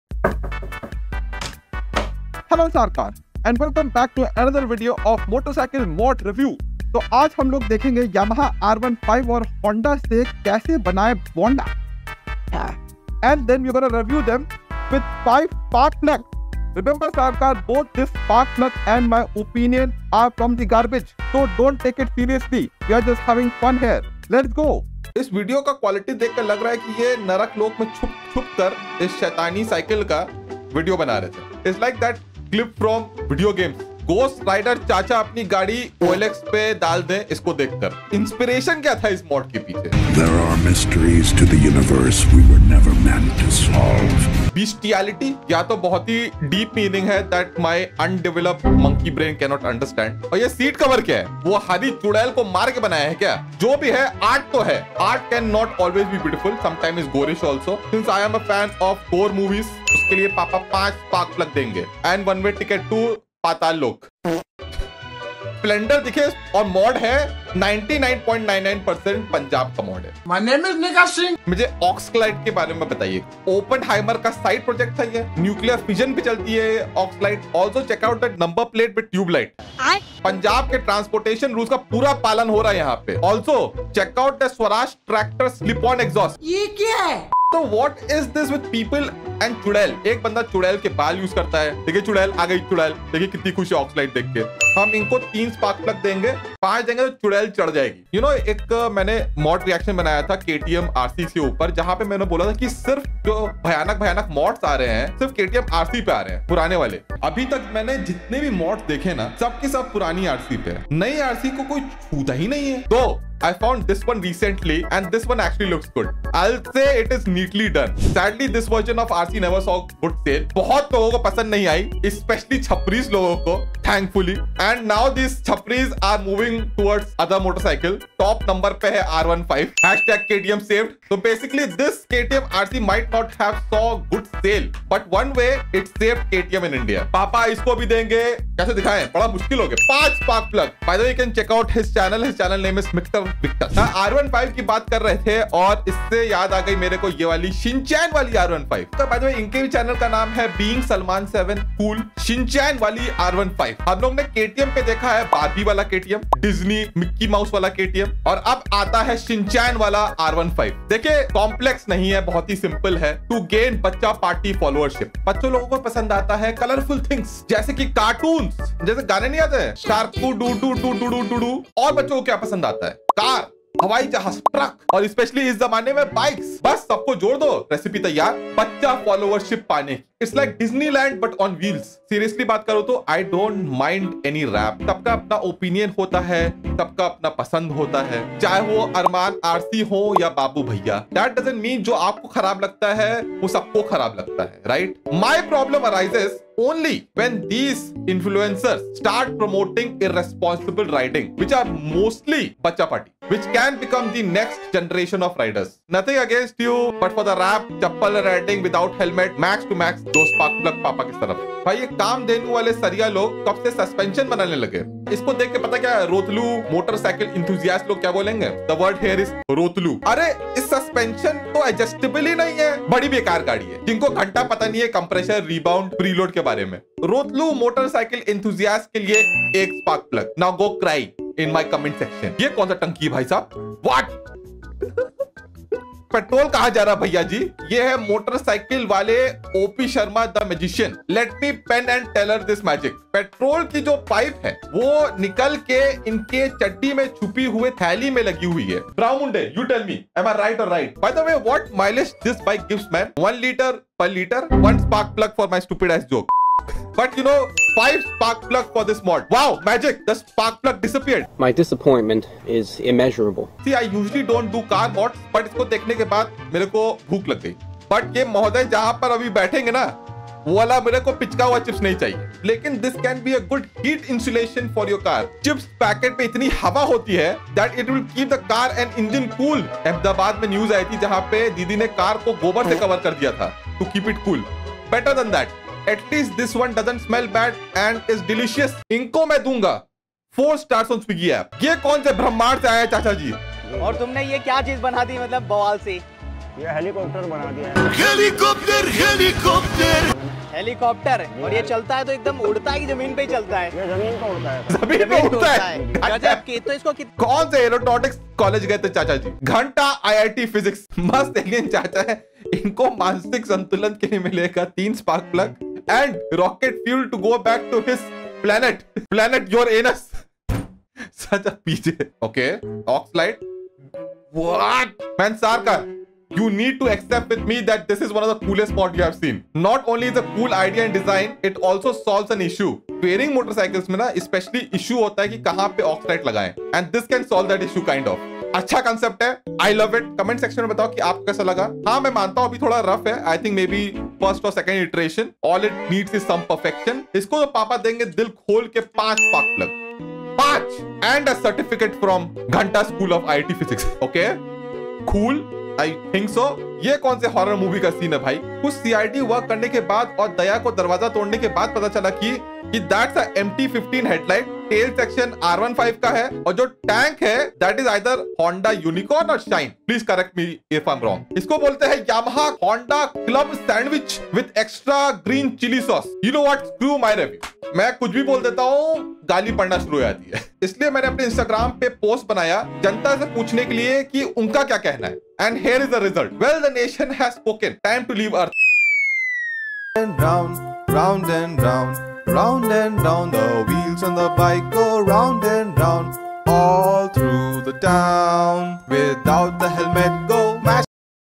This Motorcycle Mods will kidnap your kids!! Yamaha R15 vs MT15 Modification
In this video we are going to review weirdest and coolest modification of motorcycles done in India and rate them from 1 to 5 spark plugs.

Motorcycle consist of
Yamaha r15
Yamaha mt15
Honda Unicorn
Honda Shine
Cool Motorcycle mods
Punjab Splendor mod
KTM RC 2023 Modified